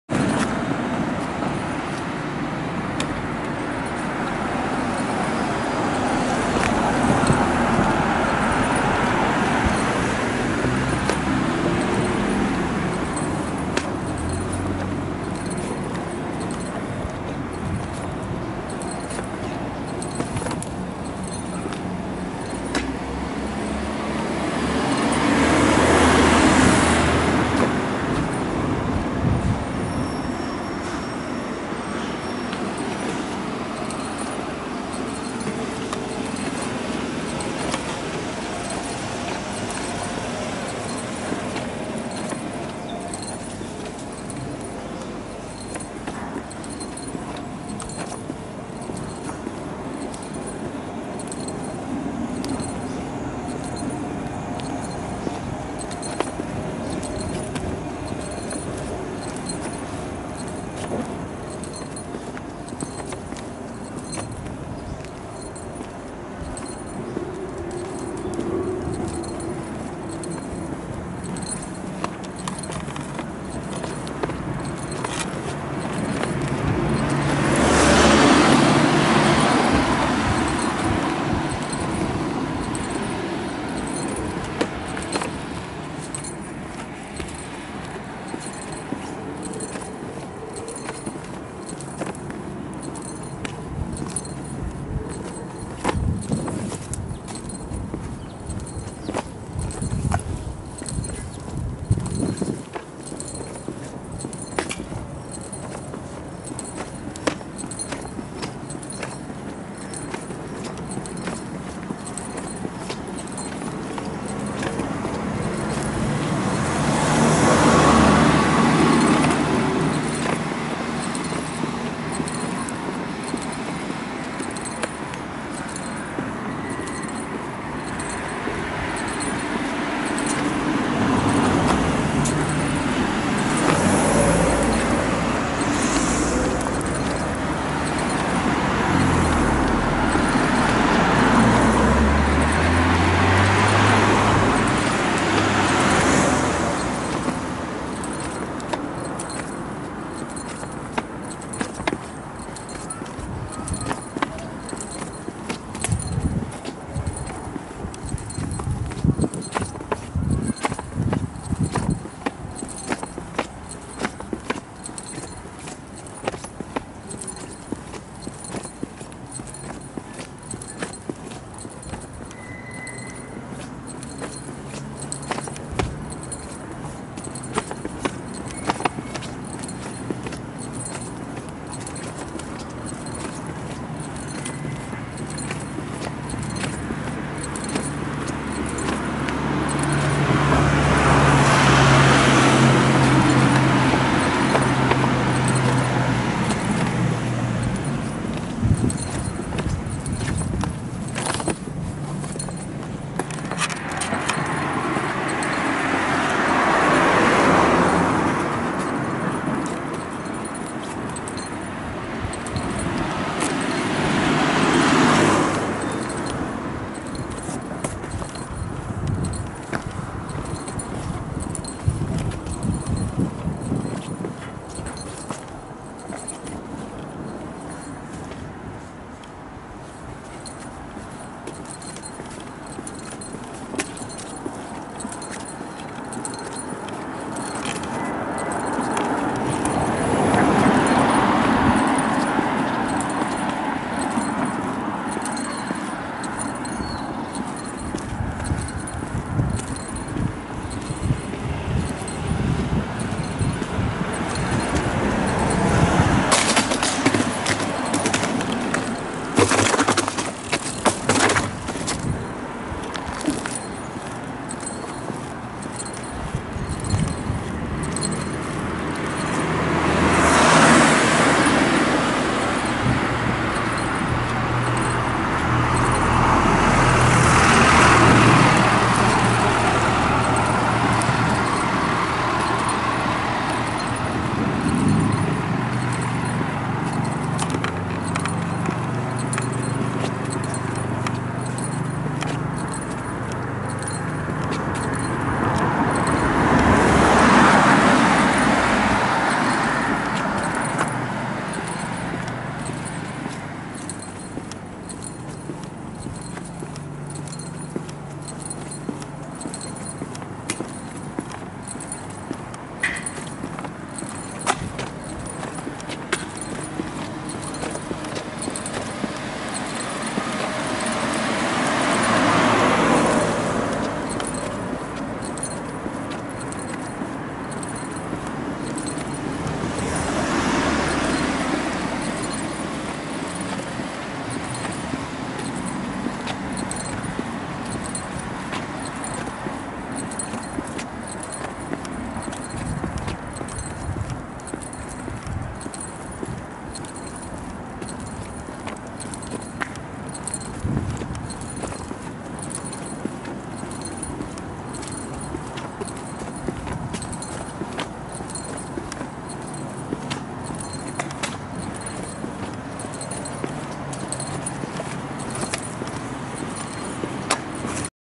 【観光地シリーズ】小田原かまぼこ通り
小田原かまぼこ通りです。
実はこの時までかまぼこ通りを知りませんでした！
たまたまJRさんのハイキングコースに入っていて歩いていたら
ありました。ここは蒲鉾屋さんがいっぱいあります。
よかったら一度お立ち寄りください！